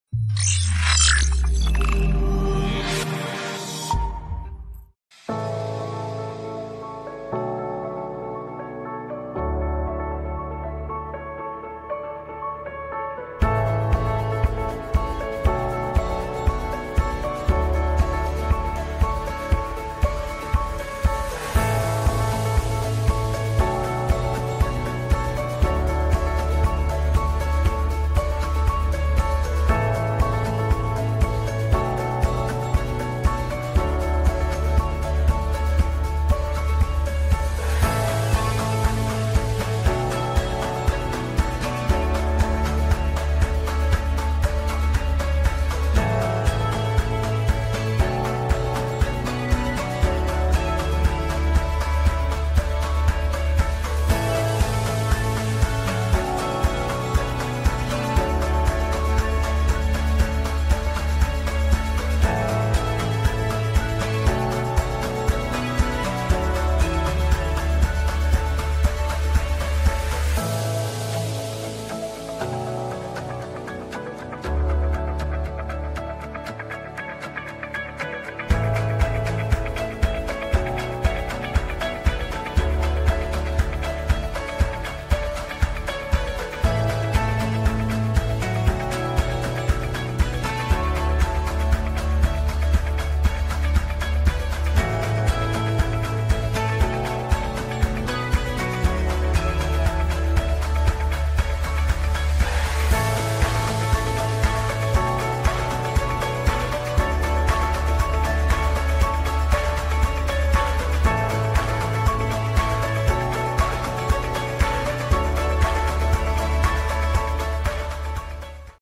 OBDSTAR X300 Classic G3 Toyota 89245-12040 Steering Angle Sensor Repair on Bench - Cardiagtool
OBDSTAR X300 Classic G3 Key Programmer with Built-in CAN FD DoIP Supports Car, E-Car, HD, Motorcycle, Marine IMMO